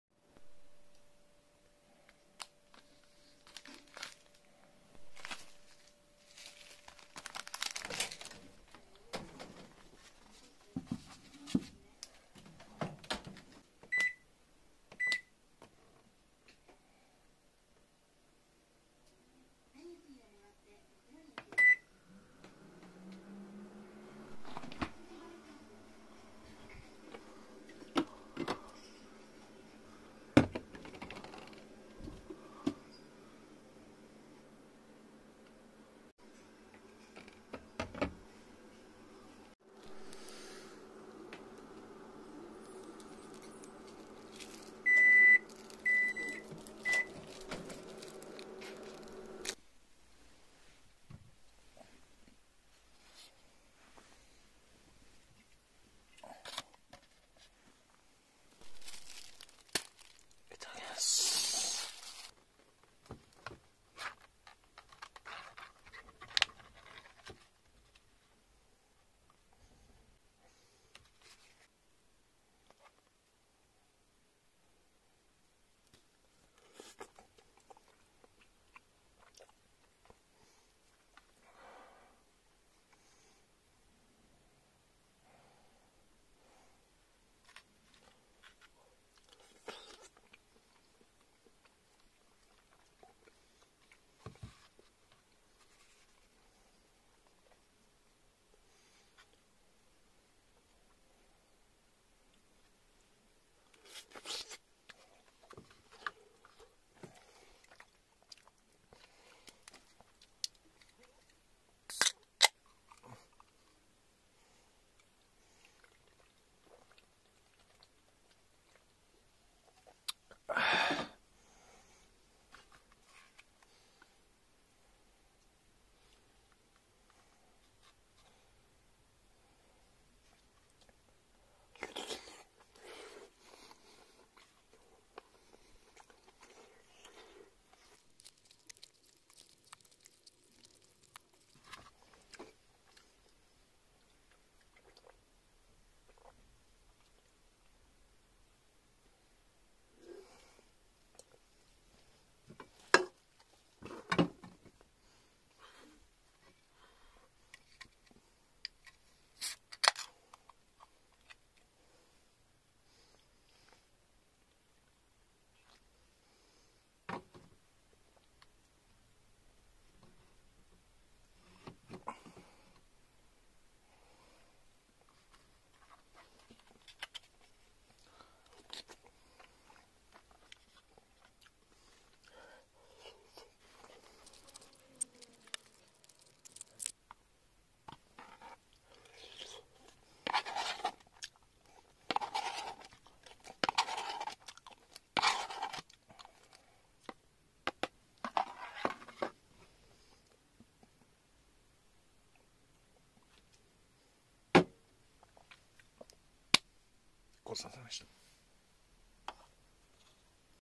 [コンビニ晩酌] セブンイレブンで北海道づくし！ごく普通の一般男性がSuage監修スープカレーを、ガラナハイボールとサッポロクラシックでキめるだけの動画
ちなみに、僕はまだ北海道行ったことありませんw